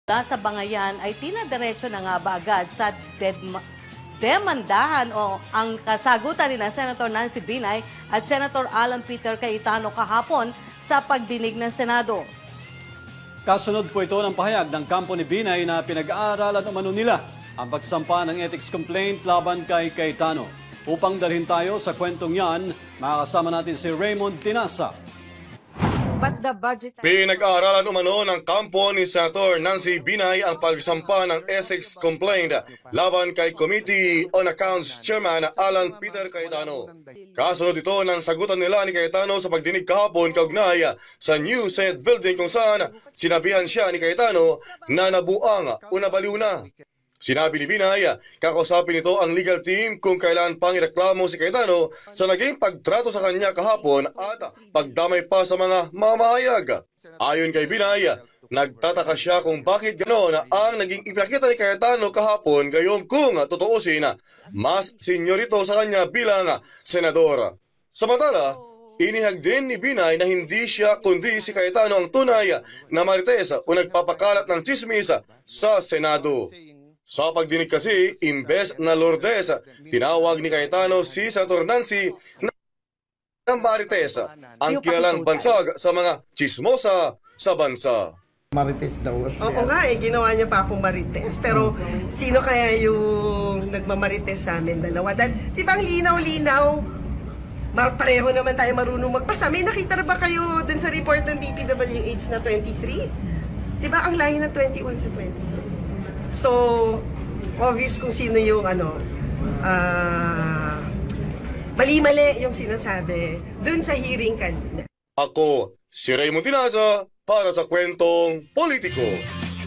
Nancy Binay, kakasuhan si Alan Cayetano?
Pinag-aaralan ng kampo ni Senator Nancy Binay ang paghahain ng ethics complaint laban kay Sen. Alan Peter Cayetano kasunod ng sagutan ng dalawa kahapon sa senate hearing patungkol sa New Senate Builduing.